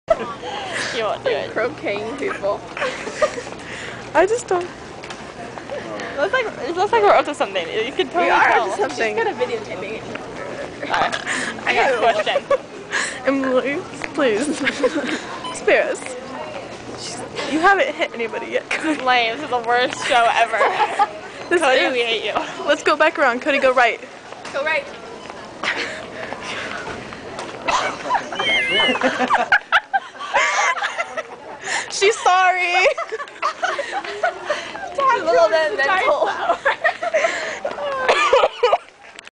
codi hits someone with a flower
my friend codi found this flower at Target and decides to swing it around as we walked in hopes she'd hit someone...which she does...in the head...as she turns the corner. no remorse lol